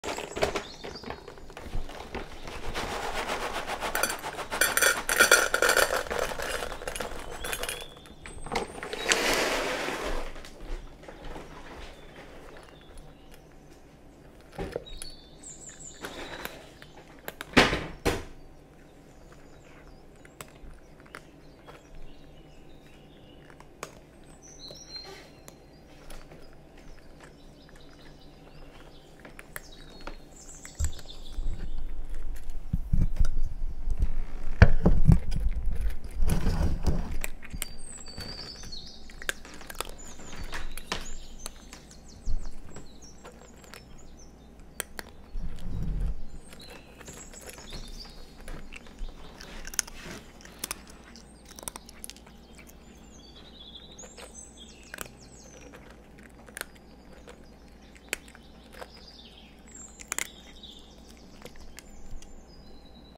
Kitty SNACK crunching in FOREST (#21)
Bengal eats kibble ASMR! Crunchy Friskies dry cat food (or Purina?) creates mouth sounds for a beautiful crackly crackle and a relaxing, calm atmosphere, this time deep in a forest! The wind and forest and eating sfx create the ultimate ambiance to unwind and fall asleep too :)